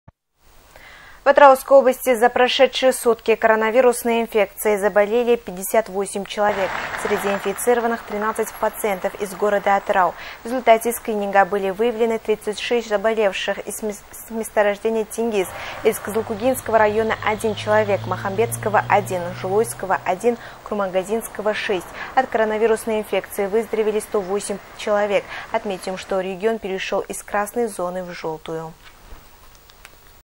ОТ КОРОНАВИРУСНОЙ ИНФЕКЦИИ ВЫЗДОРОВЕЛИ 108 ЧЕЛОВЕК
В Атырауской области за прошедшие сутки коронавирусной инфекцией заболели 58 человек. Среди инфицированных - 13 пациентов из города Атырау. В результате скрининга были выявлены 36 заболевших из месторождения Тенгиз. Из Кызылкогинского района - 1 человек, Махамбетского - 1, Жылыойского - 1, Курмангазинского - 6. От коронавирусной инфекции выздоровели 108 человек. Отметим, что регион перешел из красной зоны в желтую.